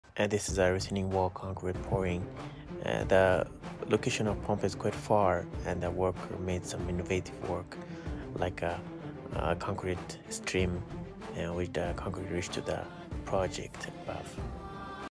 Innovation brings efficiency to the project
Innovation is the practical implementation of ideas that result in the introduction of new goods or services or improvement in offering goods or services. ISO TC 279 in the standard ISO 56000:2020 defines innovation as "a new or changed entity realizing or redistributing value".